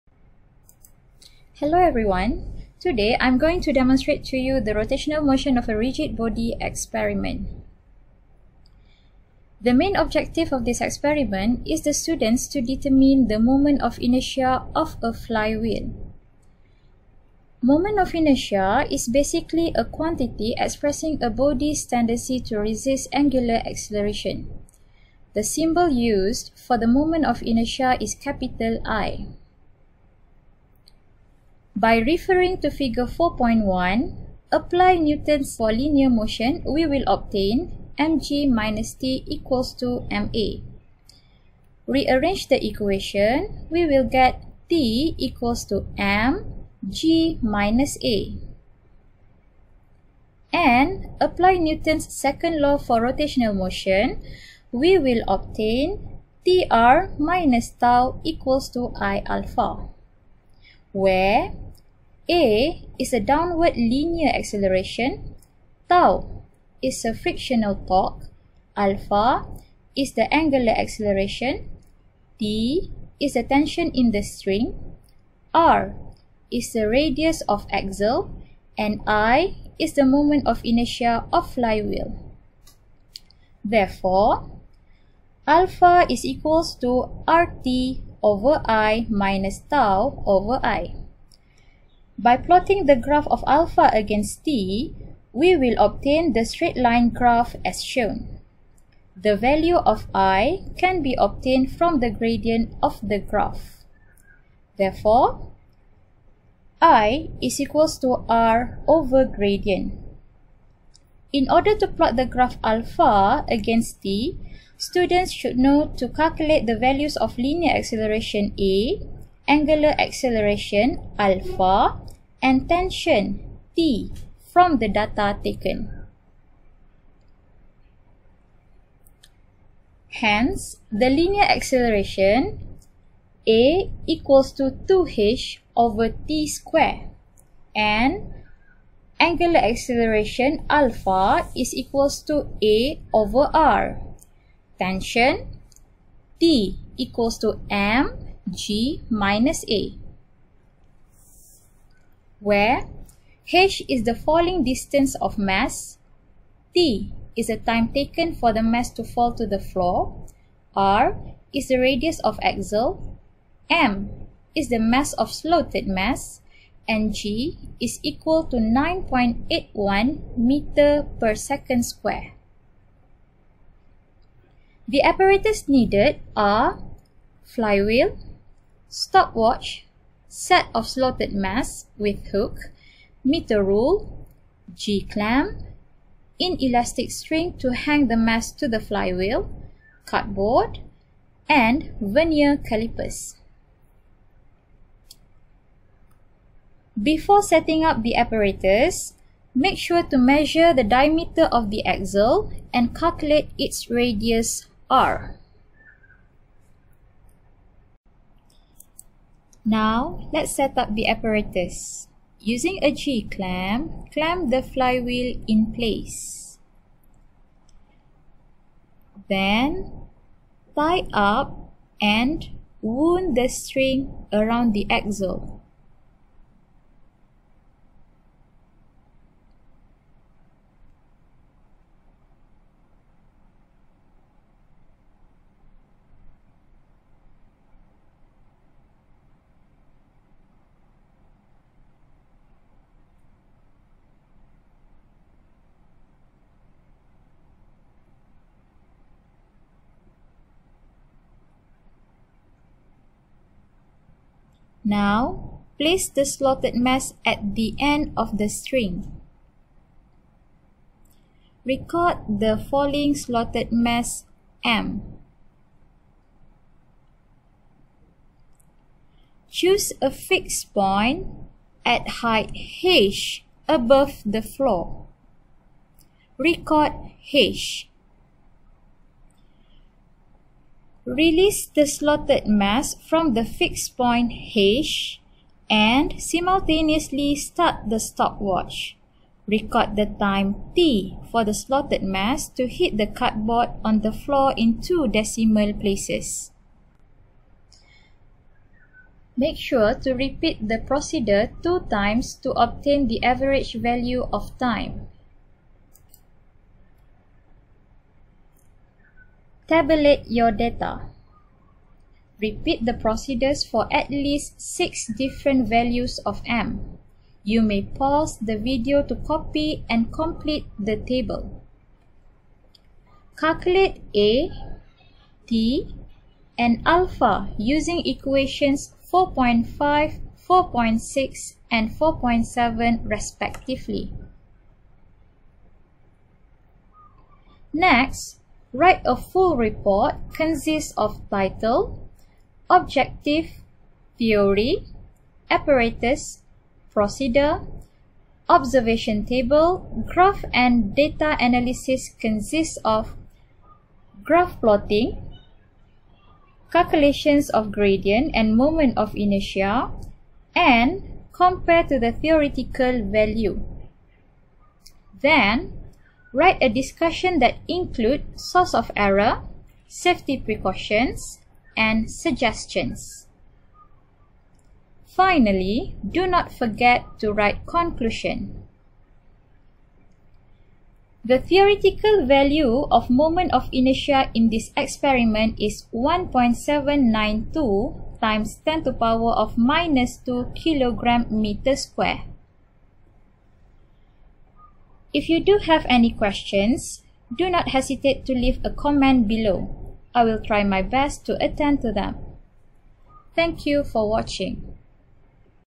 SP 015 Experiment 4: Rotational Motion of a Rigid Body (with data)
ERATA for the Theoretical value of moment inertia of the flywheel given at minute 6:21 in video :
I theoretical = 1.792 x 10 −3 kg m2